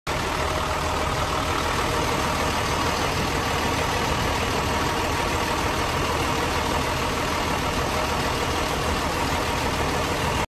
1995 FORD F800 For Sale
1995 FORDD F800 VERSALIFT BUCKET 5.9 DIESEL ENGINE RWD MANUAL TRANSMISSION .34FT WORKING HEIGHT RUNS DRIVES OPERATES LOCATED IN MORRISVILLE PA